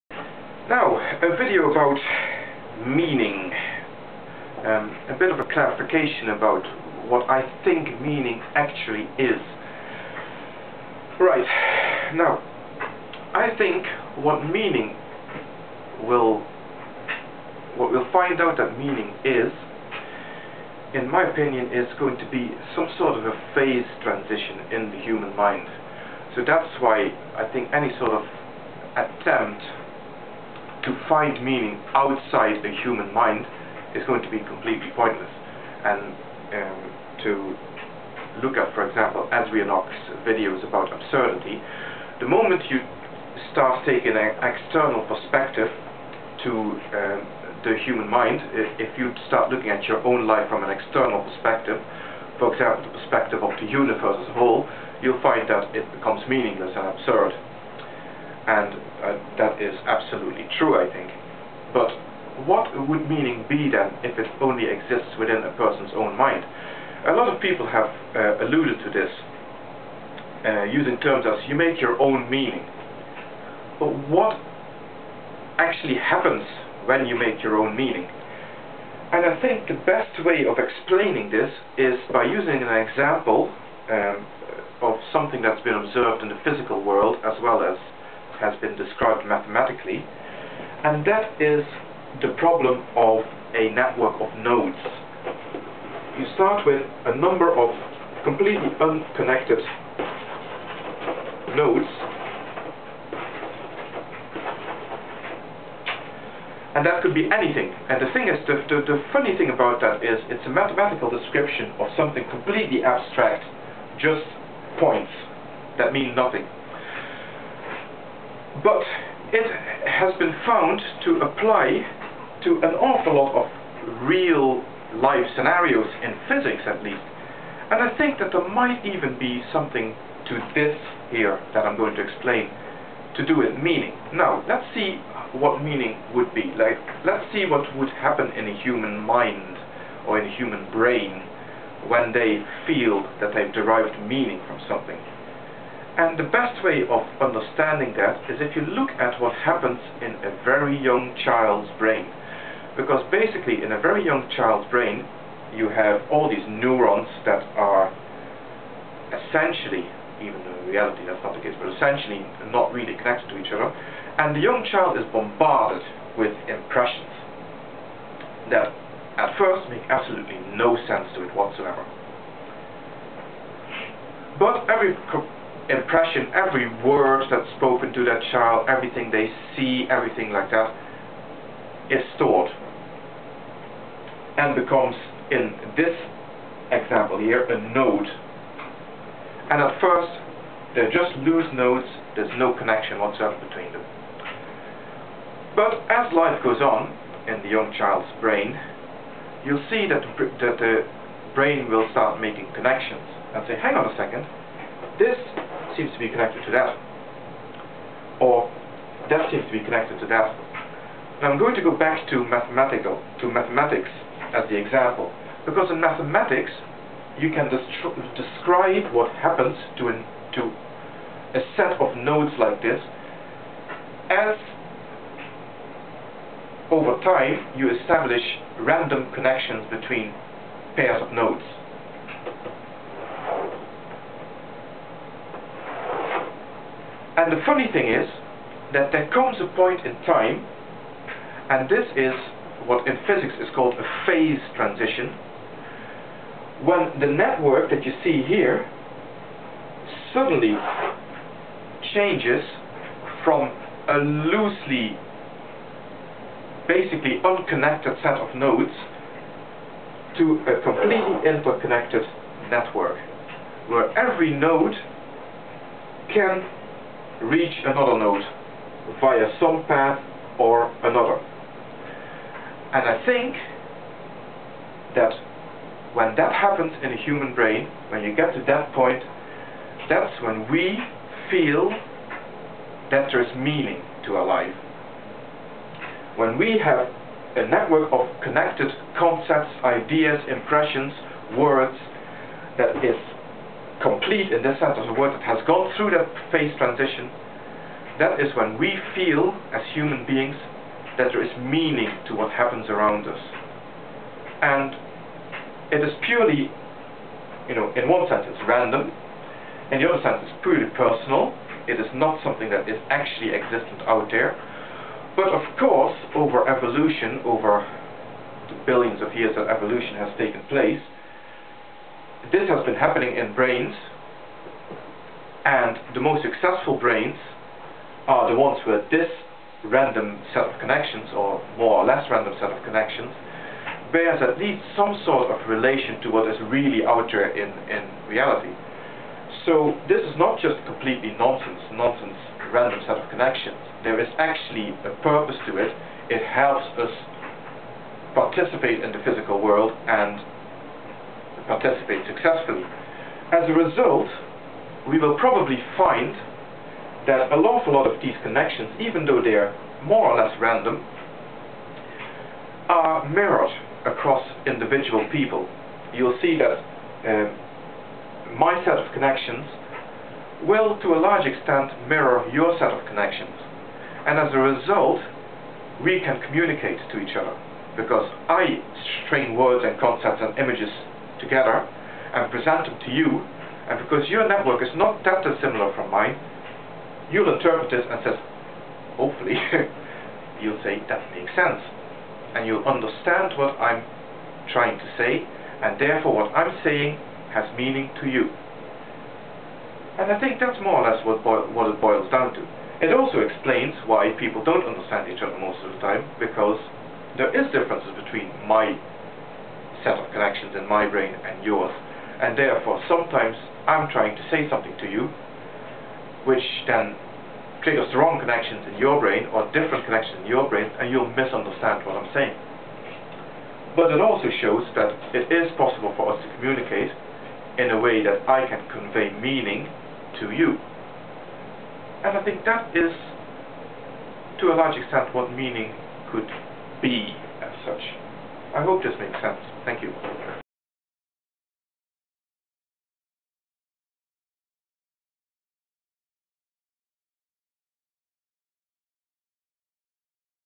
What is "meaning"?
An exploration of what I think happens when human beings sense "meaning" in the world around them.